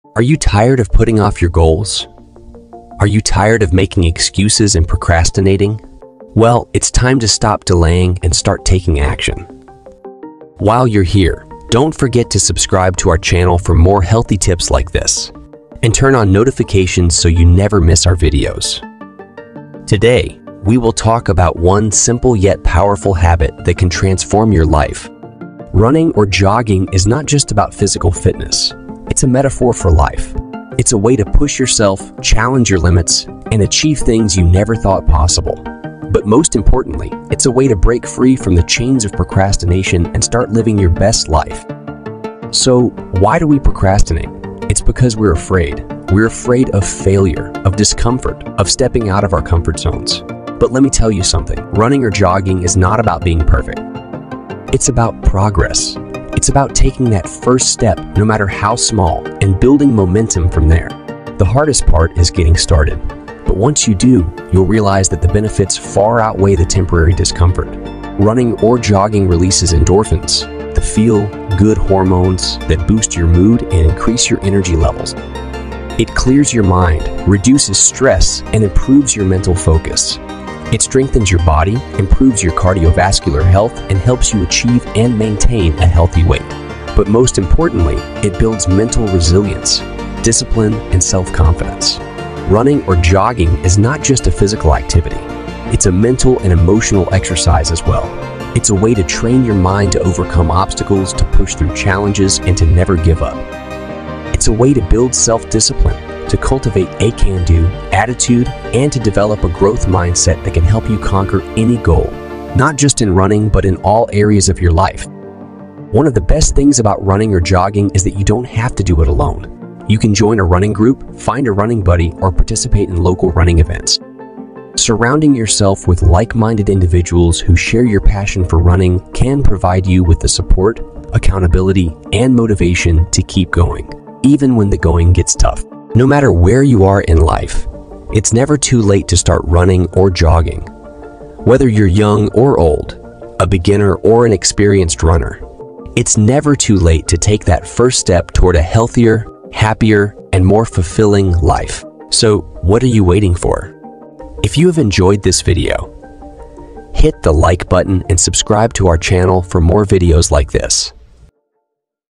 From Couch to Champion - The Unstoppable Power of Running for Motivation and Transformation!
Are you ready to go from couch to champion? In this captivating YouTube video, we'll explore the awe-inspiring power of running as a catalyst for motivation and transformation. Join us on an incredible journey as we follow the stories of ordinary people who have unlocked their true potential through the simple act of putting one foot in front of the other. Witness their triumphs, struggles, and breakthroughs as they push past their limits, overcome obstacles, and achieve extraordinary feats. From the initial steps on the couch to the victorious finish line, this video will show you how running can be a life-changing tool for personal growth, mental resilience, and physical well-being. Get ready to be inspired, motivated, and transformed by the unstoppable power of running!